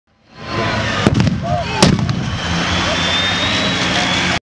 Boom!
Short clip of the Tank 'battle' at Wartime in the Vale Military Show.